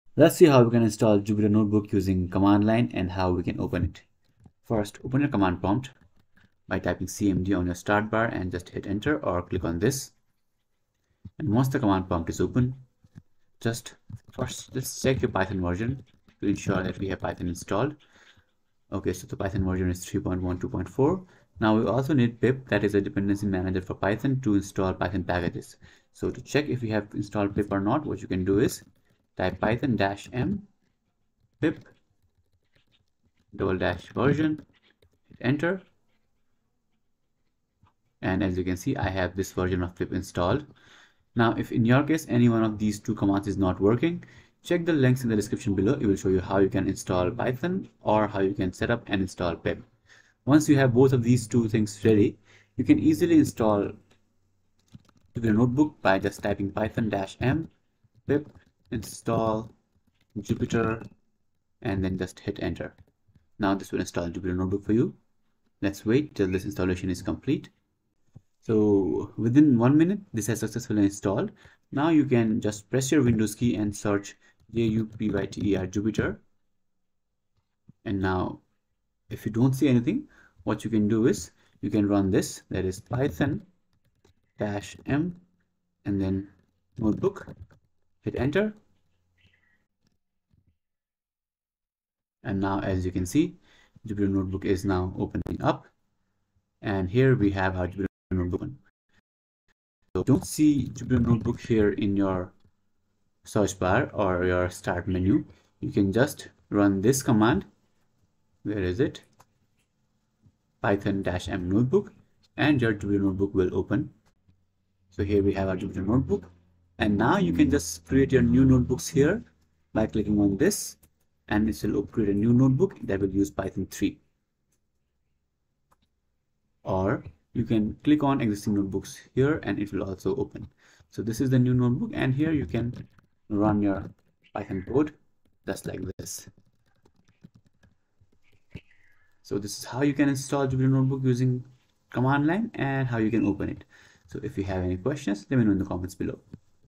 How to Install Jupyter Notebook Using CMD (2024)
In this video, I'll show you how you can install Jupyter Notebook using cmd (command prompt) easily.
Jupyter Notebook is a popular web based tool that allows you to code in python on your web browser. It provides interactive interface, visualization and more.

Steps to Install Jupyter Notebook using command prompt (CMD)
1. Open Command Prompt
2. Install Jupyter Notebook using PIP: pip install notebook
3. Open Jupyter Notebook: python -m notebook

By the end of this video, you'll have installed jupyter notebook easily using PIP and command prompt(CMD).